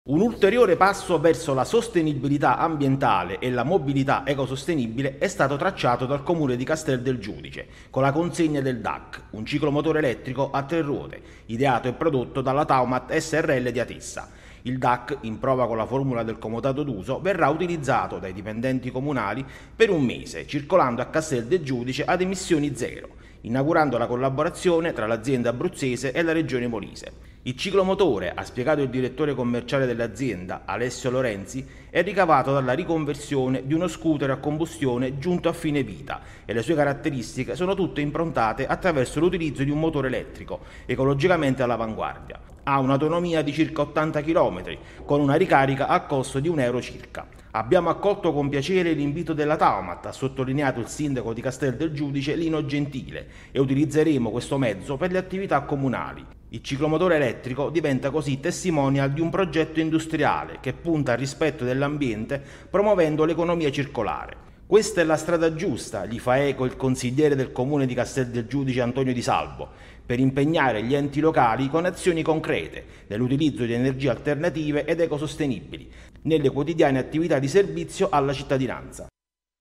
𝗨𝗡 𝗨𝗟𝗧𝗘𝗥𝗜𝗢𝗥𝗘 𝗣𝗔𝗦𝗦𝗢 𝗩𝗘𝗥𝗦𝗢 𝗟𝗔 𝗠𝗢𝗕𝗜𝗟𝗜𝗧𝗔' 𝗘𝗖𝗢𝗦𝗢𝗦𝗧𝗘𝗡𝗜𝗕𝗜𝗟𝗘
Castel del Giudice compie un ulteriore passo verso la sostenibilità ambientale e la mobilità ecosostenibile